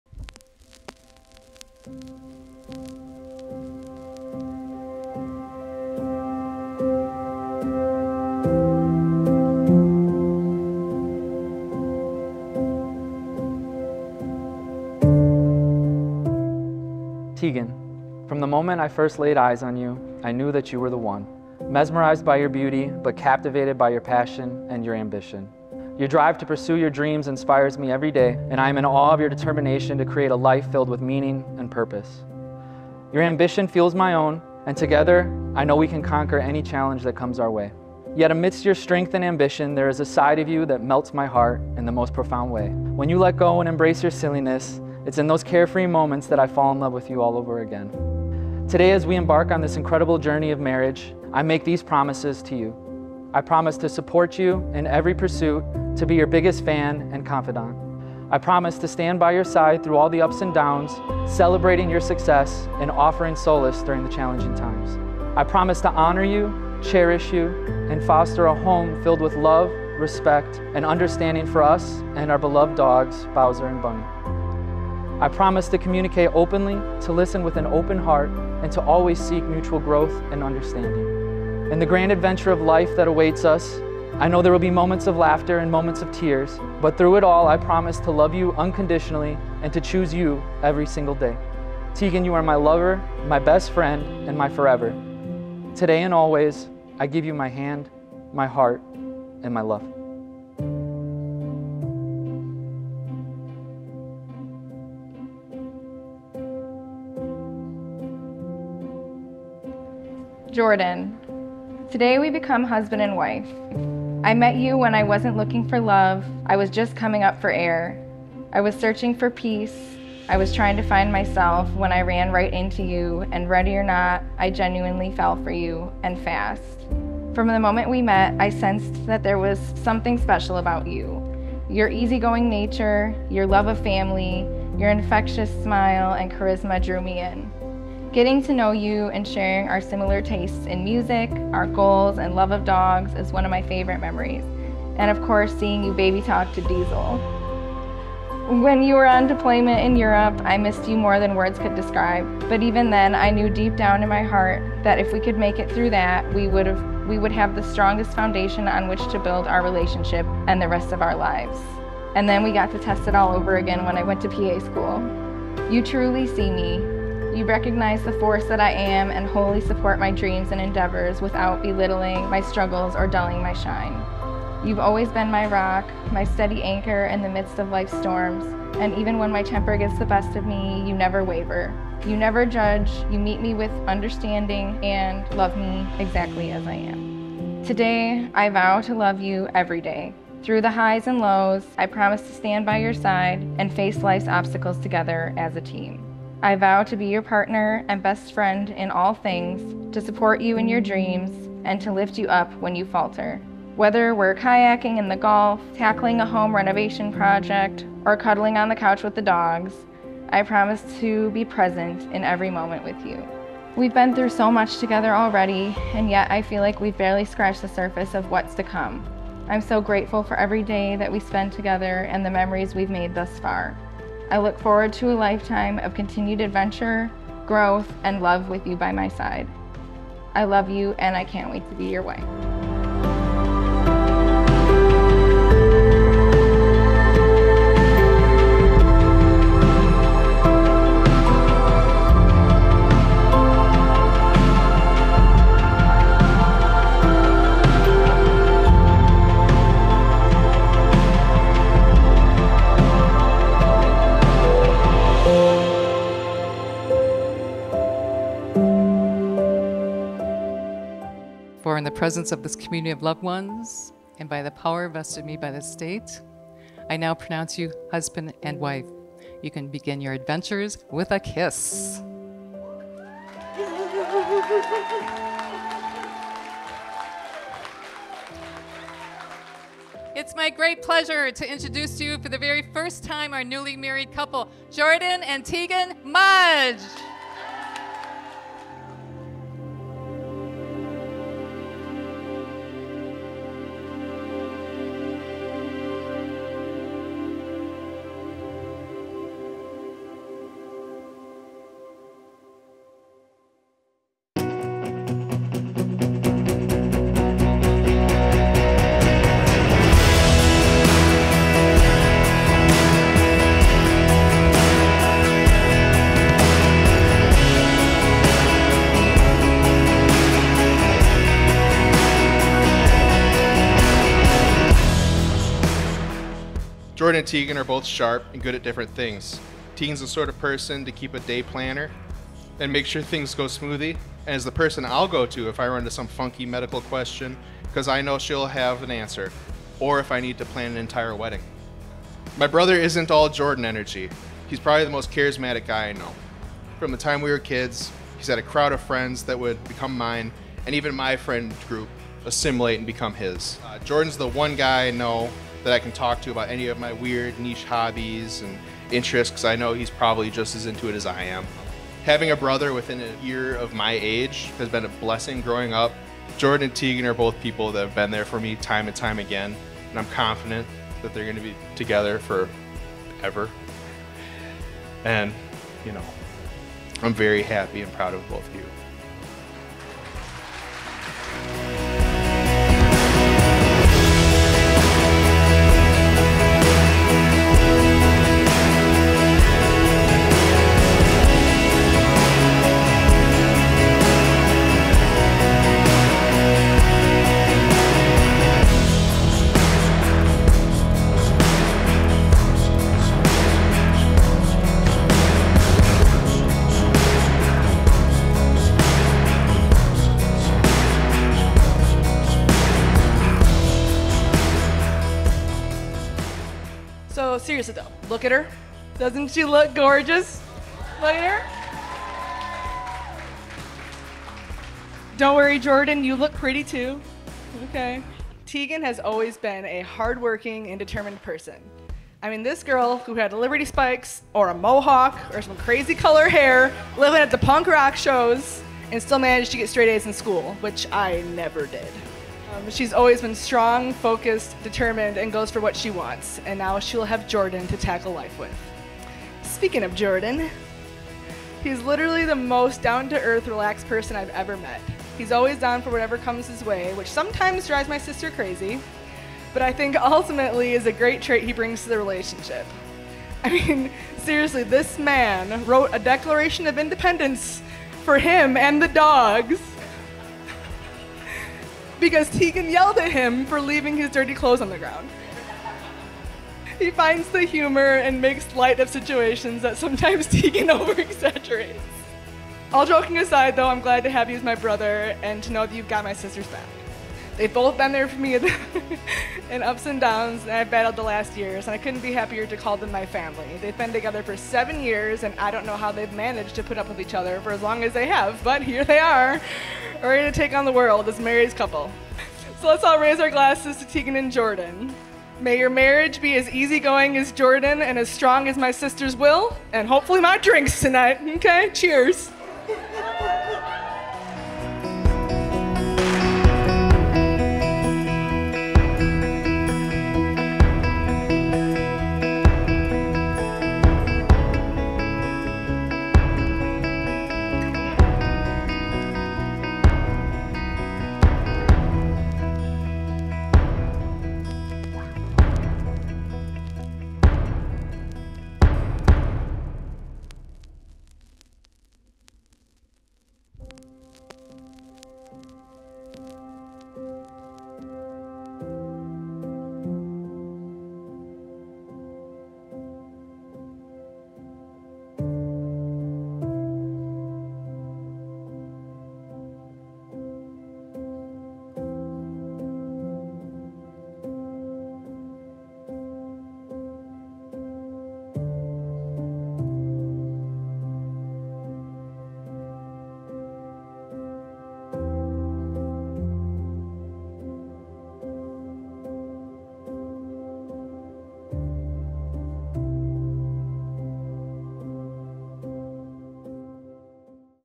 Teagan & Jordan Wedding Film | Kenosha, WI | Irv22.com
I have no words.. One of my favorite times capturing their special day.

Shot on Sony FX6, paired with the Sigma 24-70 f2.8, Atomos Ninja V and the Zhiyun Crane 3s as the main.
Supported by the Sony FX3, Sony A7IV and good 'ol trusty Sony A7RIII.
Sound: GODOX MoveLink M2 Wireless Lav mics. - They're so-so.
And Tascam Portacapture X6